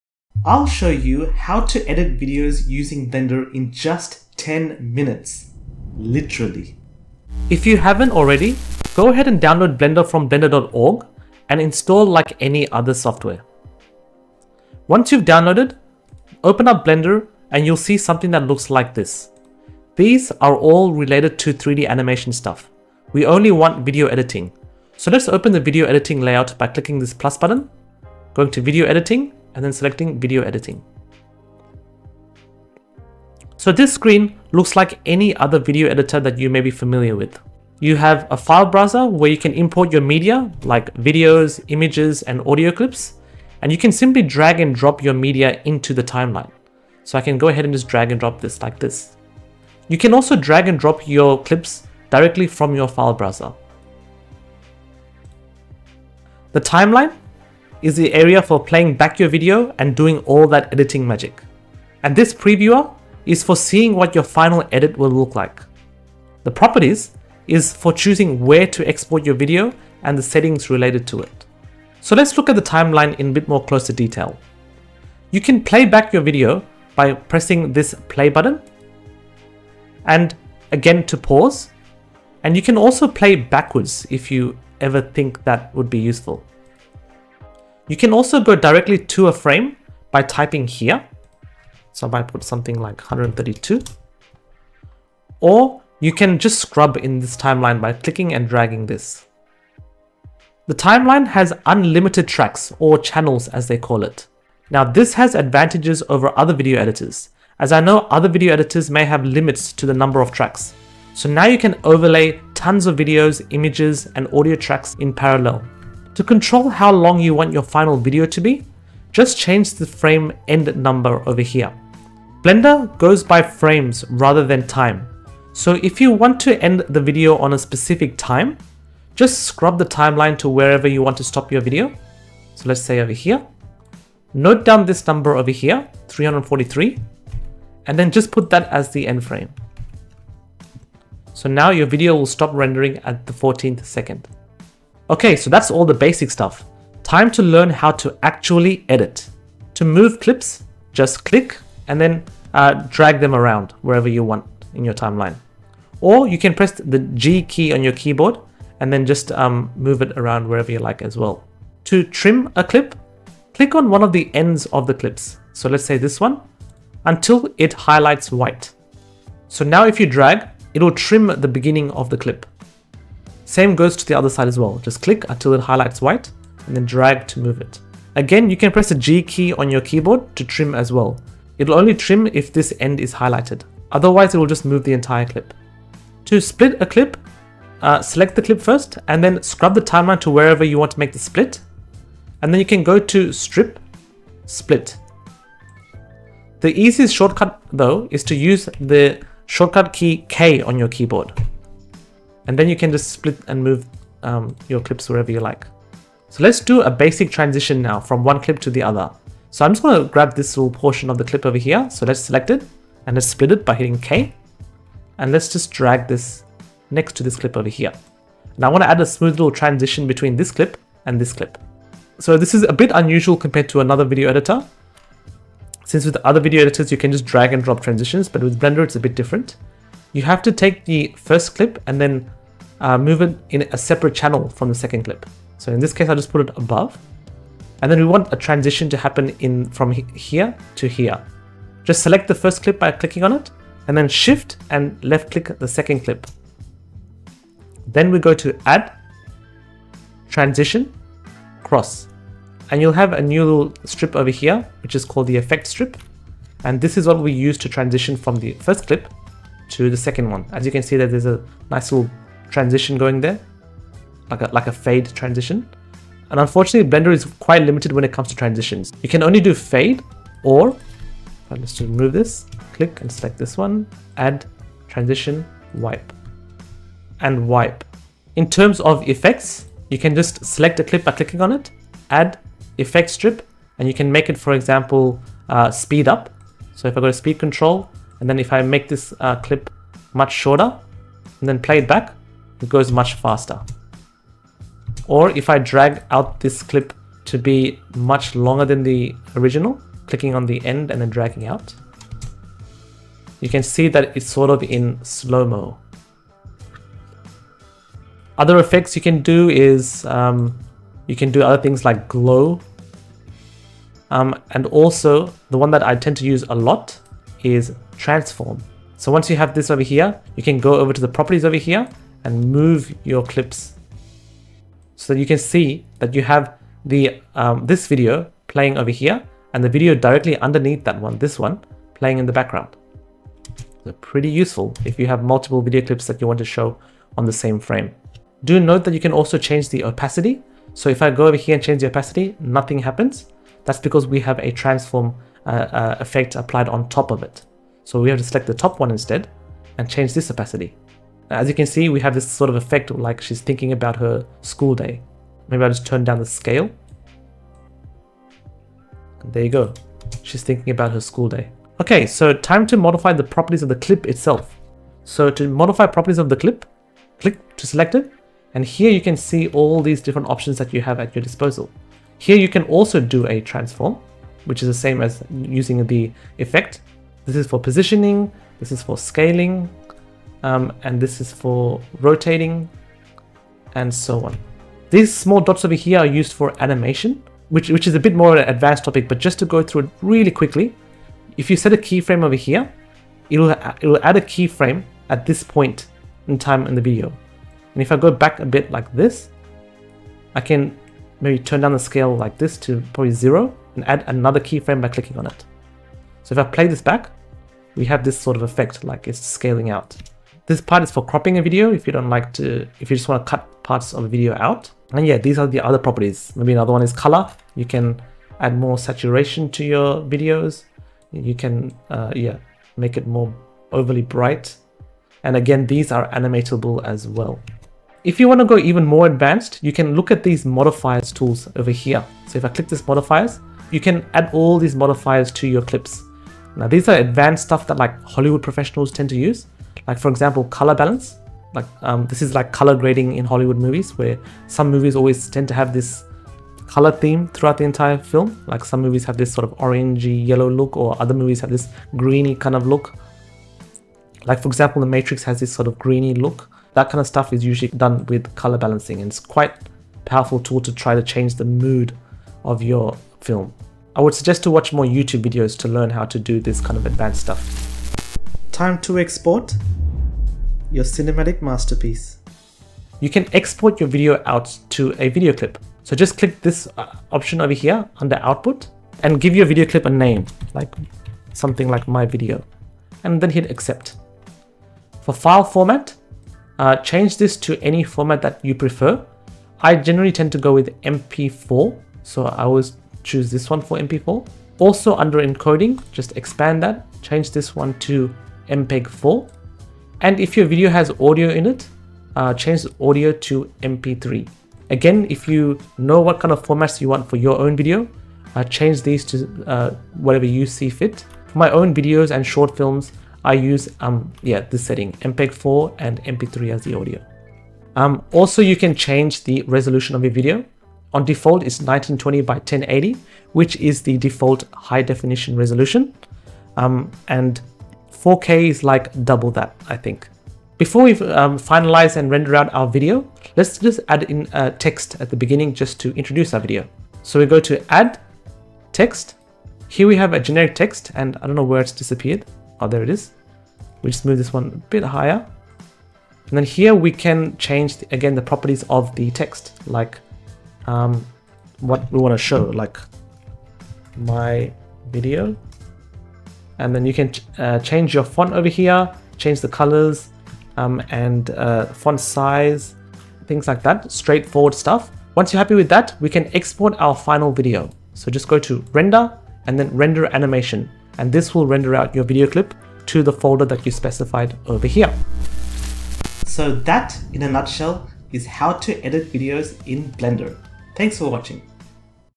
Video Editing With Blender In 10 Minutes (Everything You Need To Know)
In this video, you will learn everything you need to know about editing video with Blender.

Easy beginner tutorial on getting started with editing videos in Blender.

00:00 Introduction
00:11 Install Blender
00:31 The Video Editing Workspace
01:11 The Timeline Editor
02:25 Editing Videos
07:48 Exporting Your Final Video
08:50 Adding Text